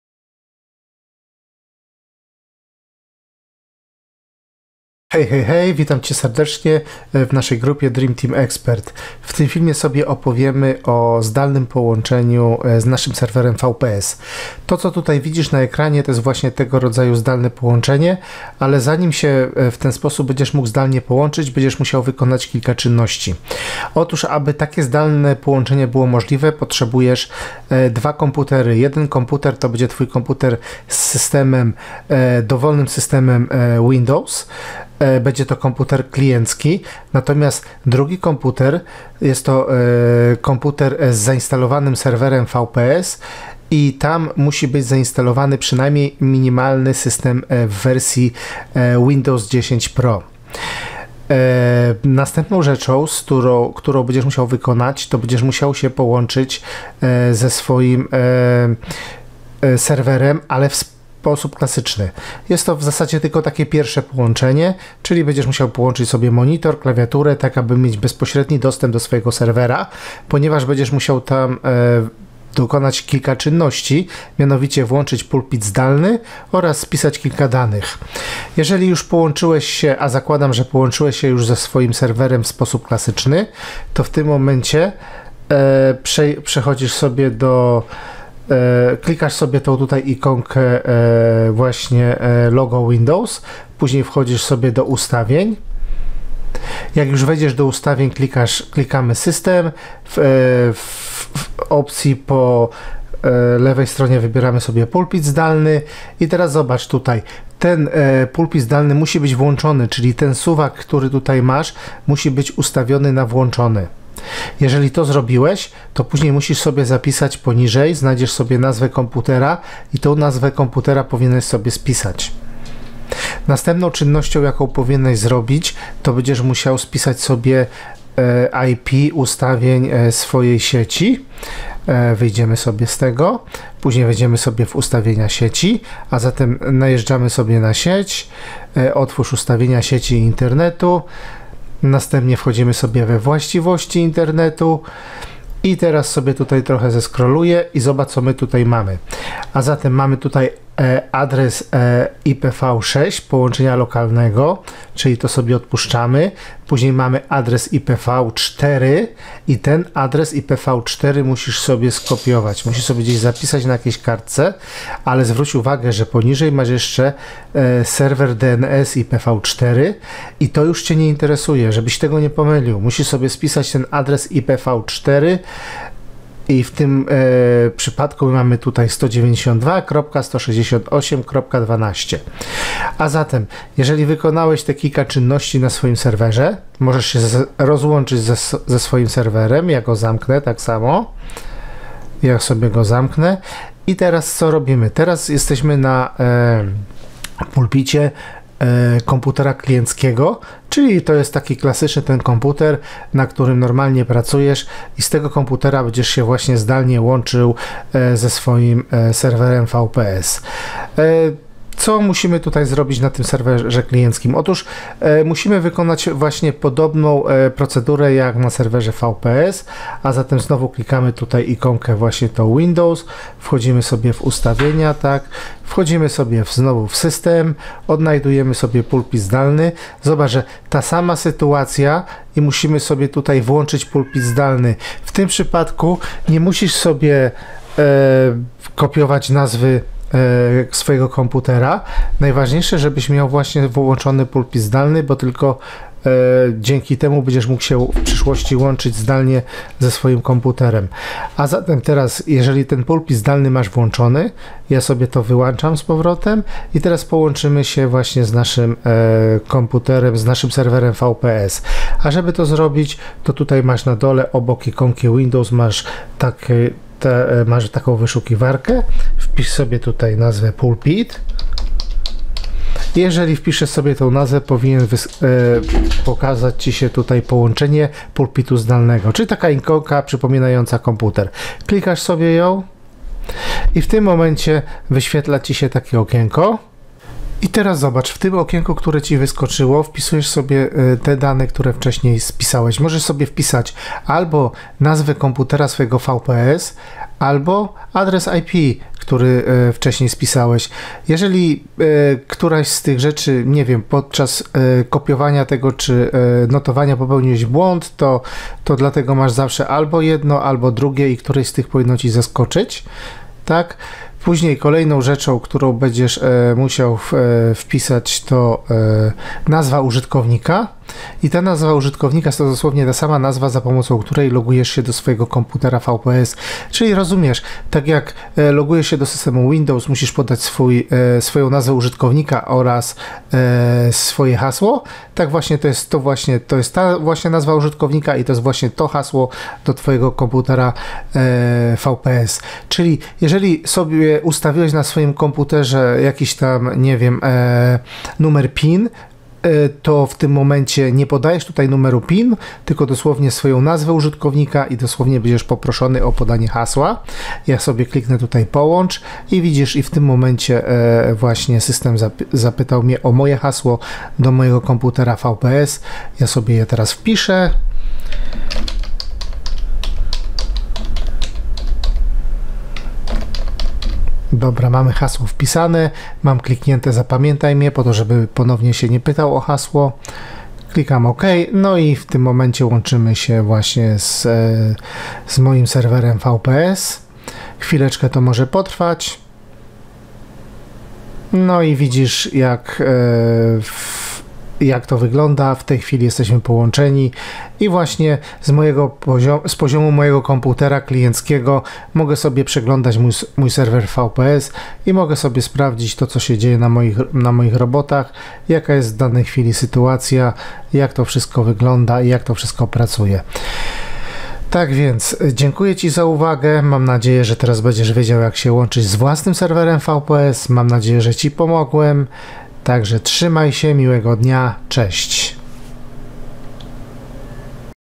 Zdalne połączenie VPS
Film opisuje w jaki sposób połączyć się z własnym serwerem VPS